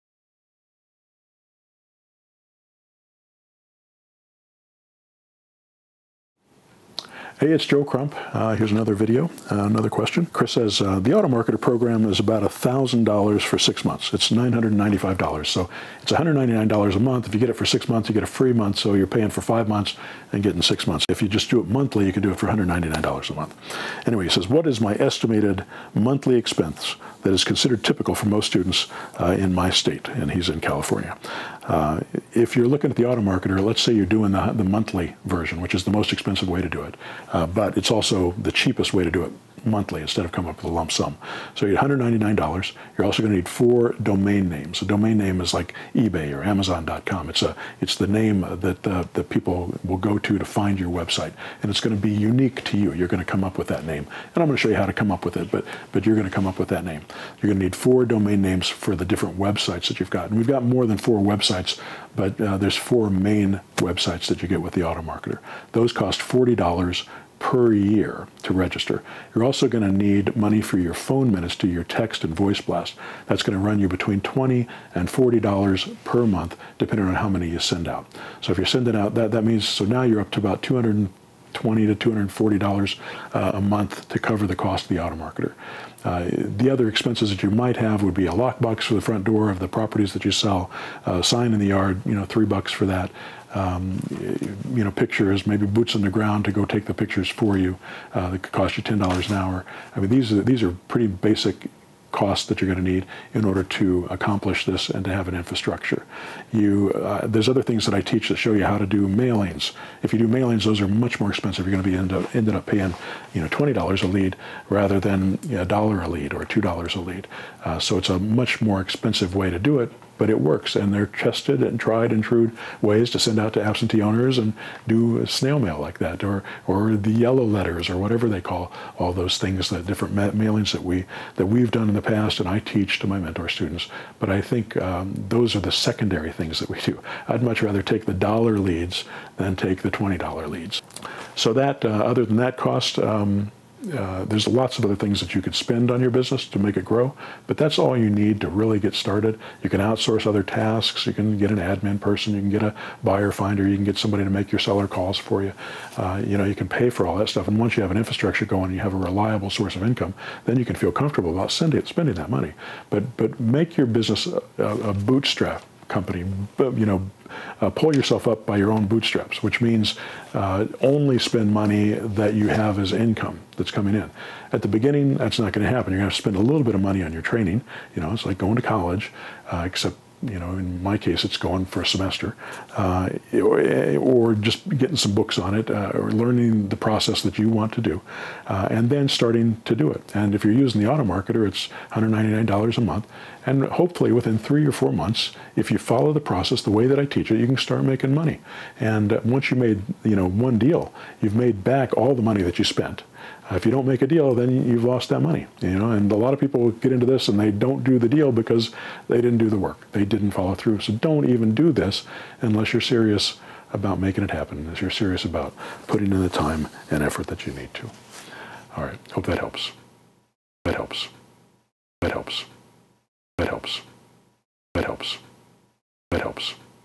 What are Typical Monthly Expenses for Students in the Automarketer Program?
Today you're going to learn what the typical monthly expenses are for students who are enrolled in the Automarketer program!

Also, most of my mentor students use this automation real estate investing marketing system that I created.